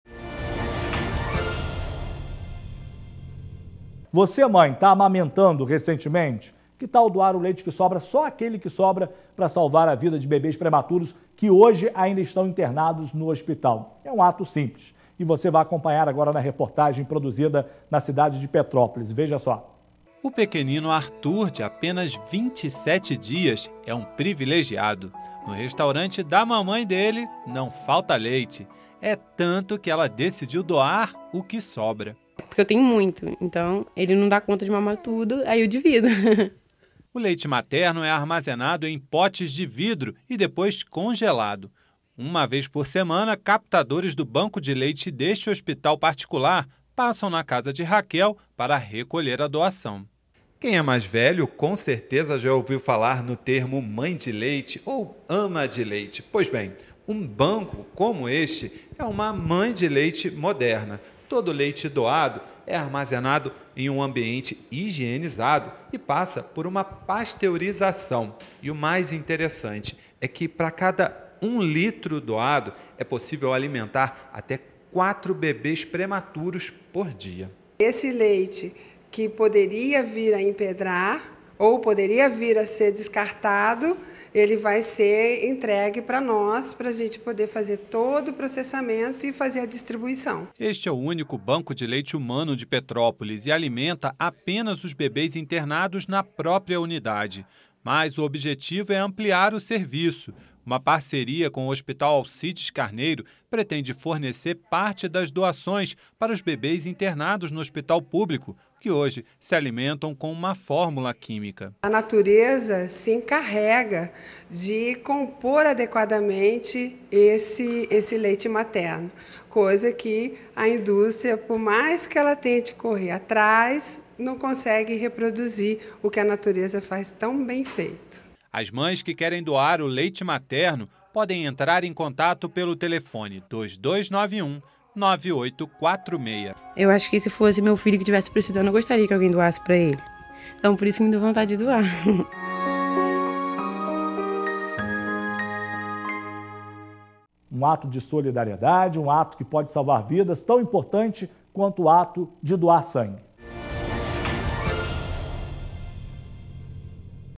Banco de leite materno ajuda no tratamento de bebês prematuros
A doação é apenas do leite excedente. Cada litro pode alimentar até quatro bebês por dia.